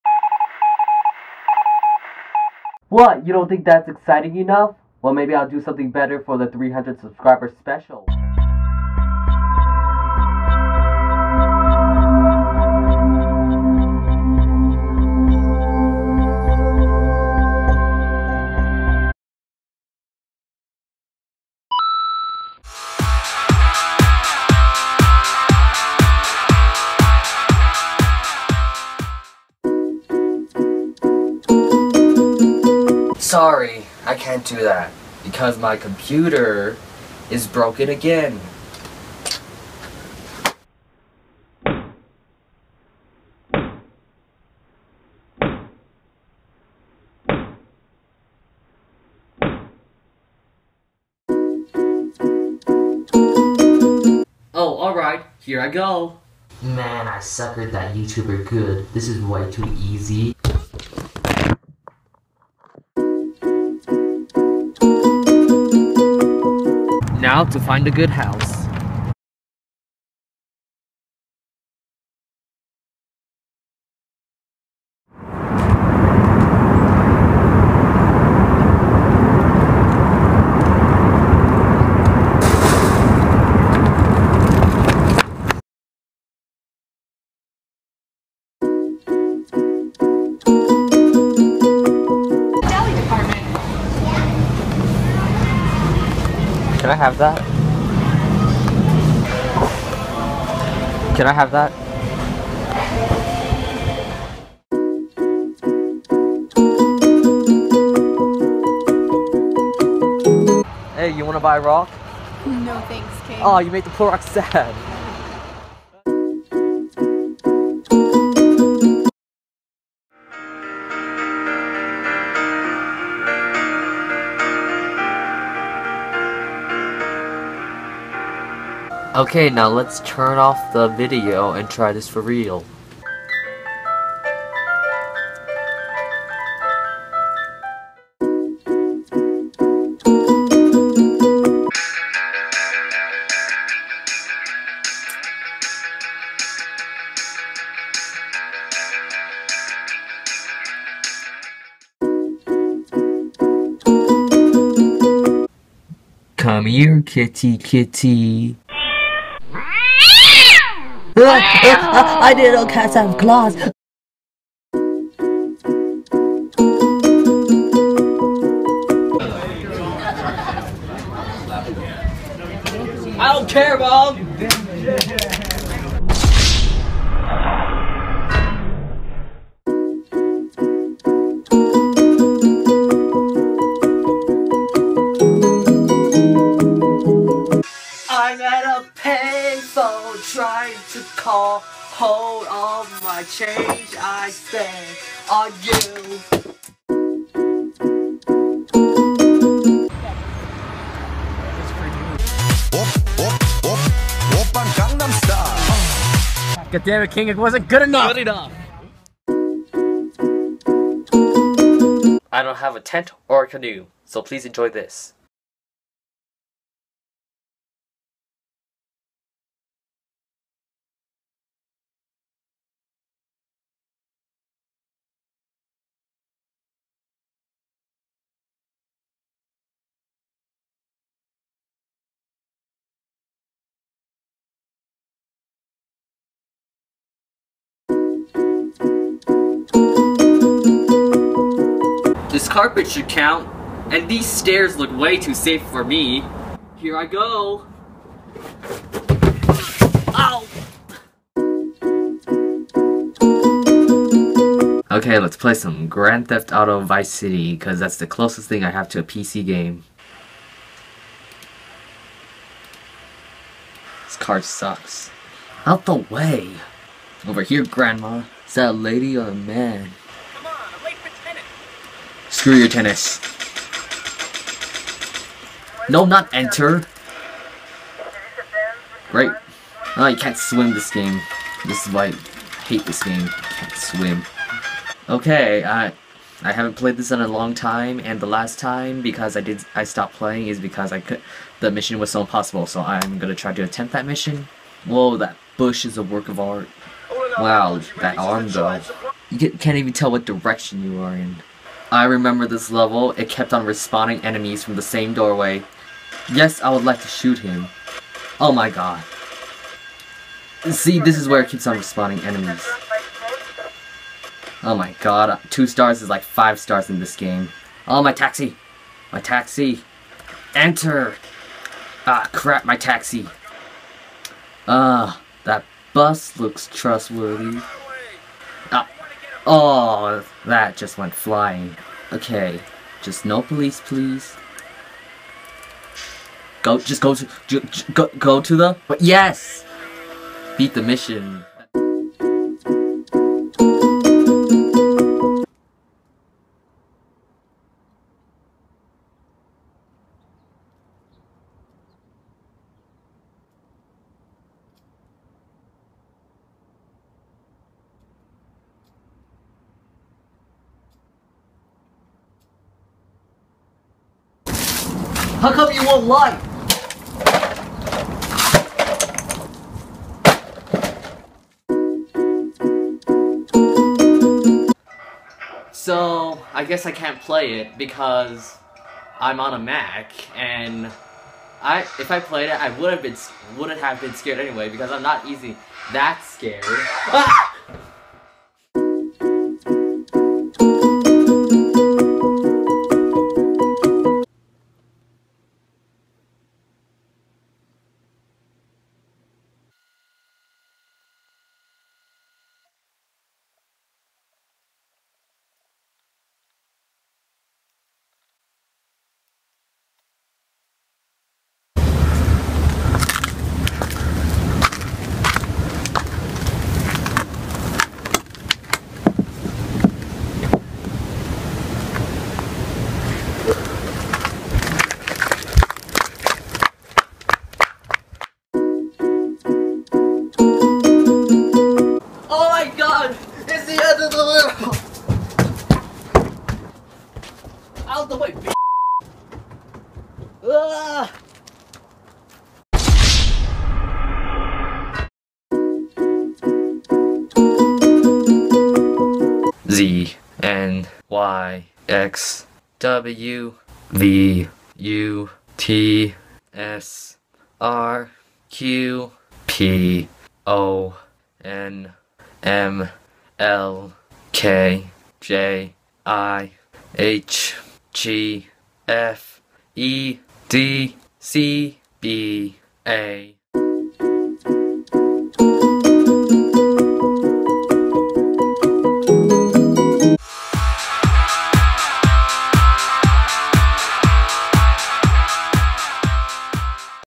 I Do Your Dares! (300 Subscriber Special!)
Cats, neighbors, pianos, and more! All in your dares that I'm gonna do!